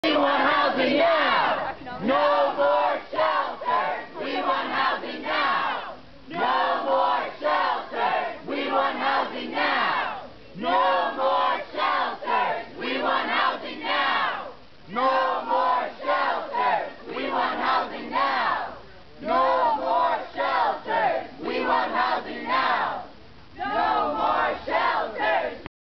Homelessness Protest at Vermont Gubernatorial Debate, Part 2
Two nights ago, if you drove by Burlington's Waterfront Theatre before the first of this year's gubernatorial debates, you probably saw a bunch of colorful signs hyping Republican Gov. Jim Douglas and his major campaign rivals, Democrat Gaye Symington and Progressive-turned-Independent Anthony Pollina. Here's what you didn't see: More than 60 homelessness advocates rallying outside the building's back entrance.